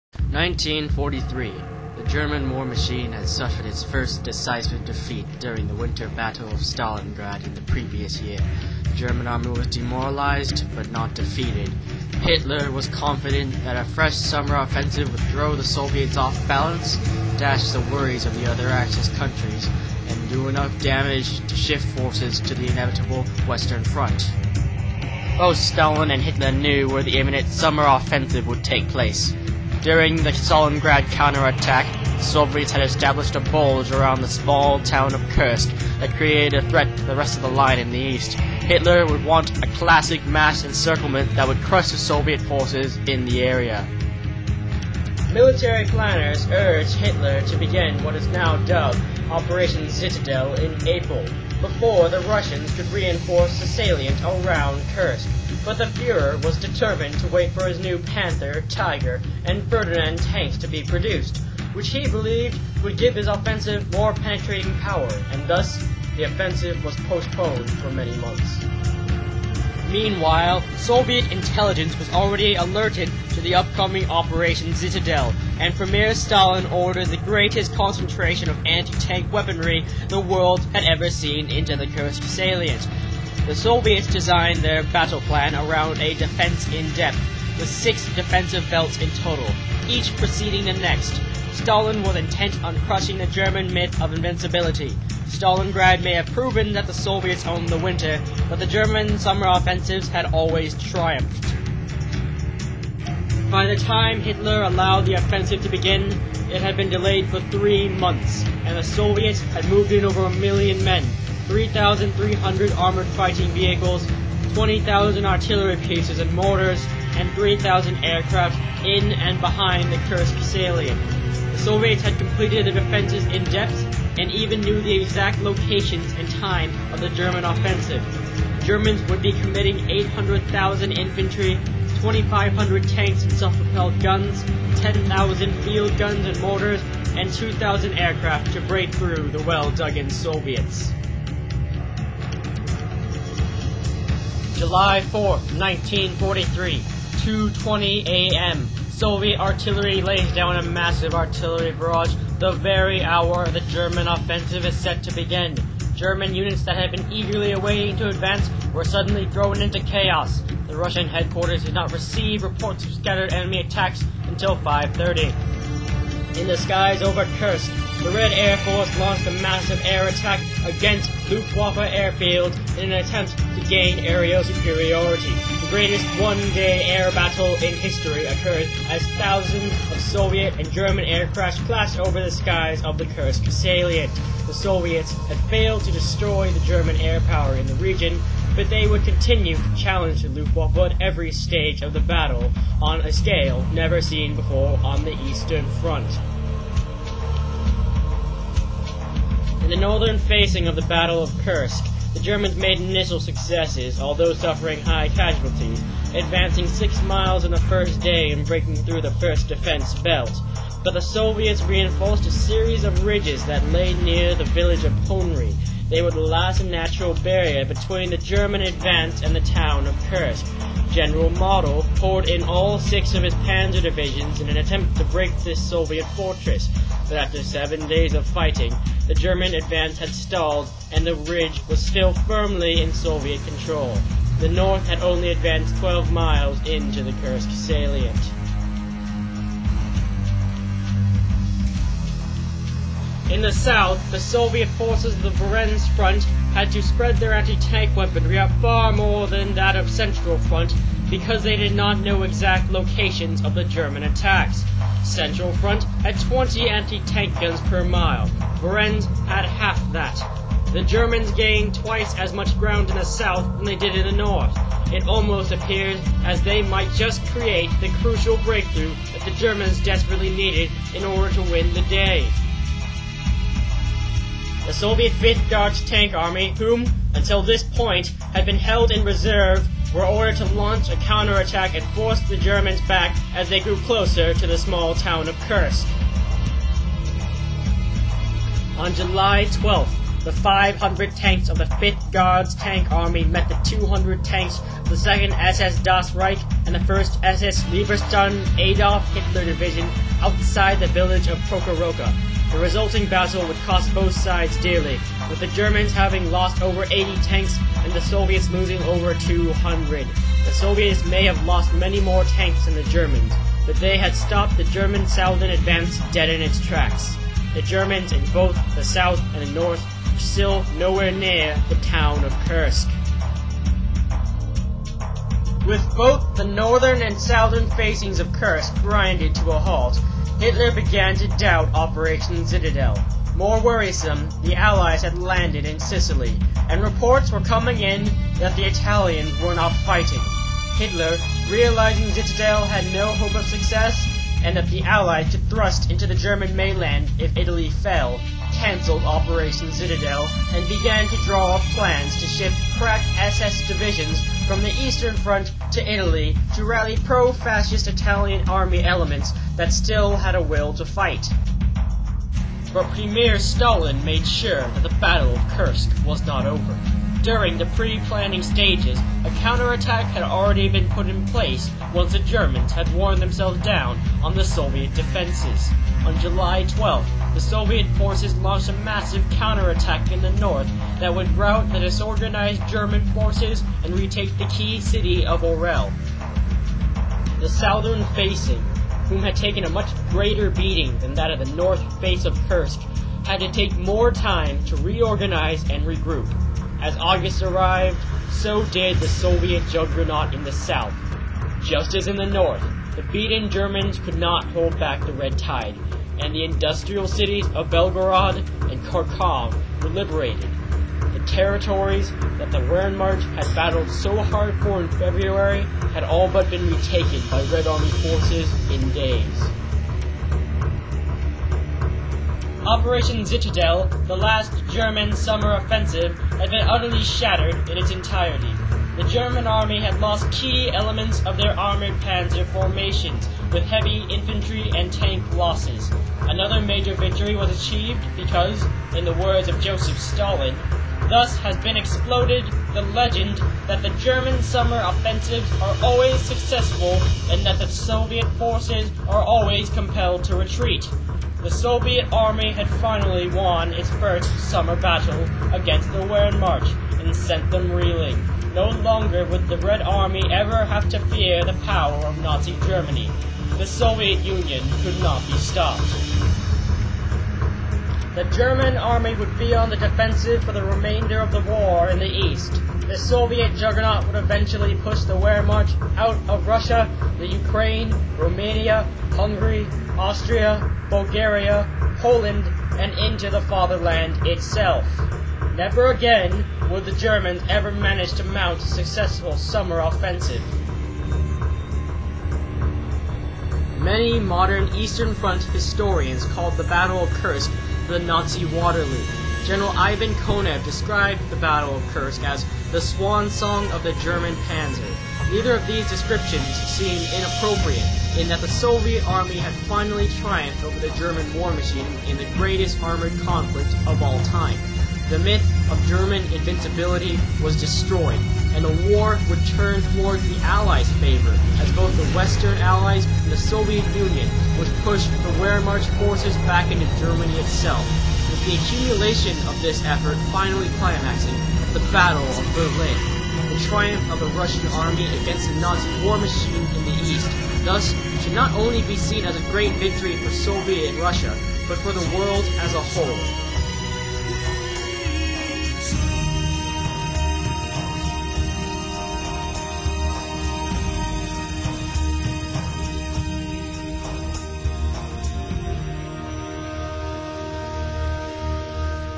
The Battle of Kursk - 2007 National History Day Documentary
A Documentary I did for National History Day about the Triumph of the Red Army over the German War Machine.